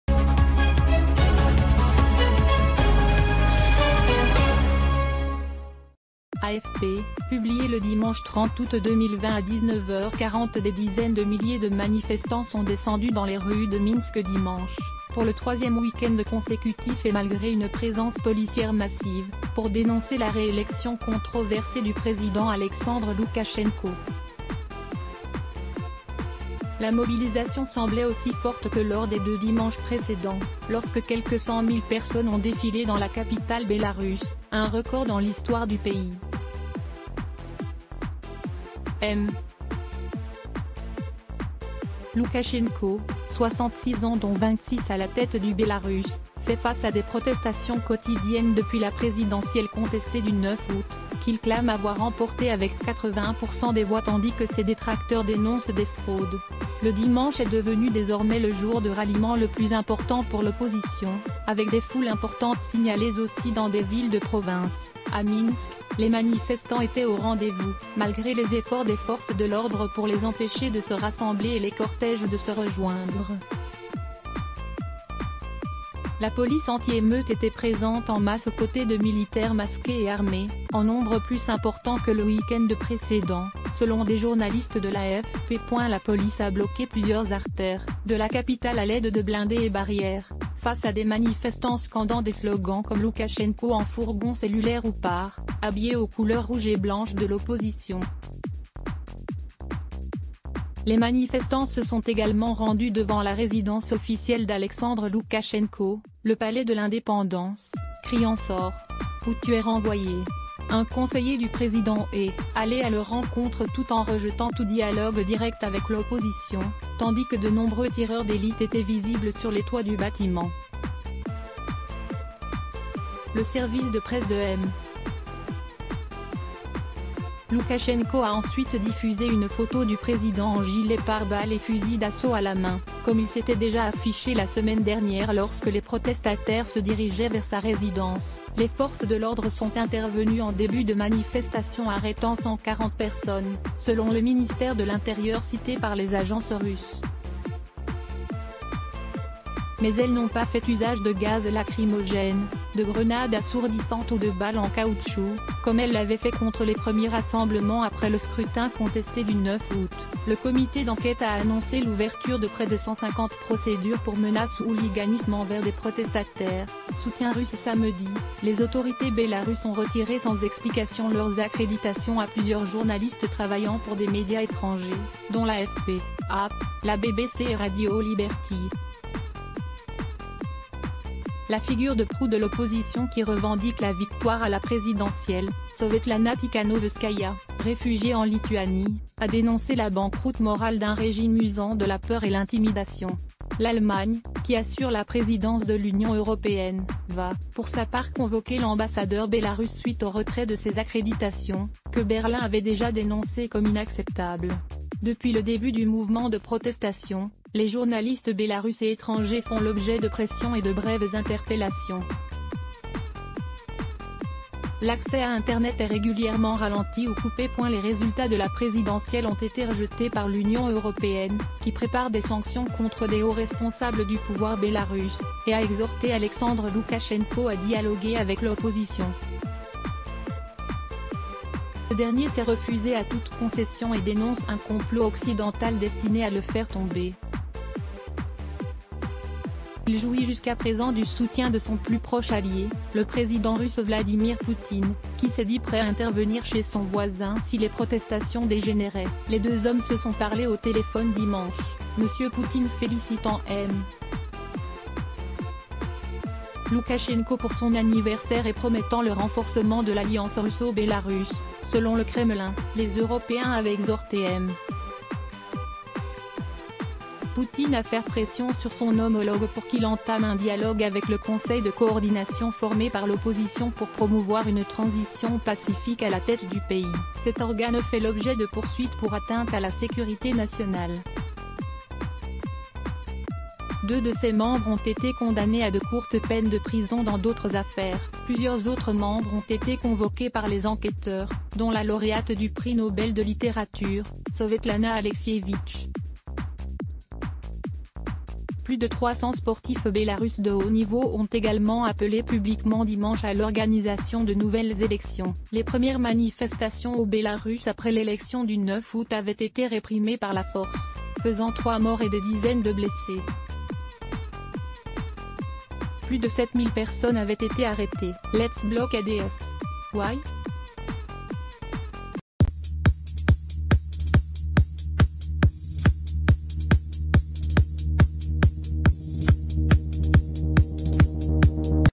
Bélarus: des dizaines de milliers de manifestants défilent encore contre Loukachenko
AFP, publié le dimanche 30 aoû...